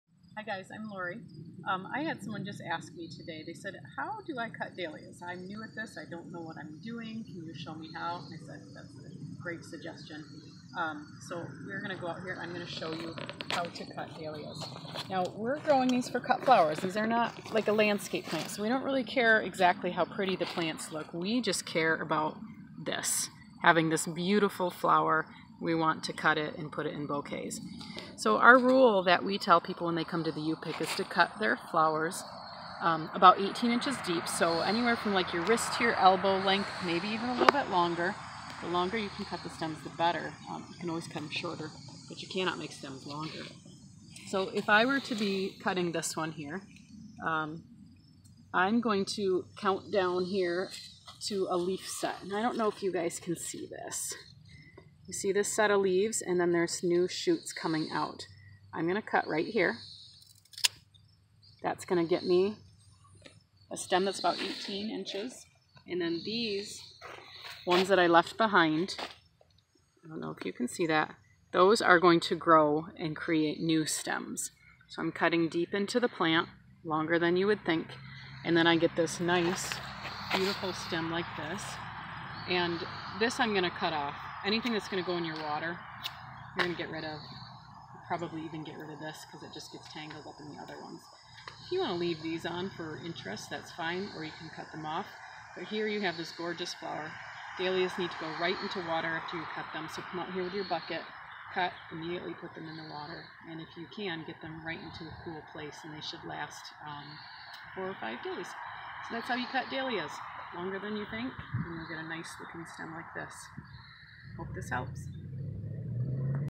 How to Cut Dahlia Blooms
Dahlias are a wonderful cut flower... and the more you cut them, the more they produce! Watch this short video to learn the proper cutting technique for dahlias. You feel like a flower cutting pro in no time!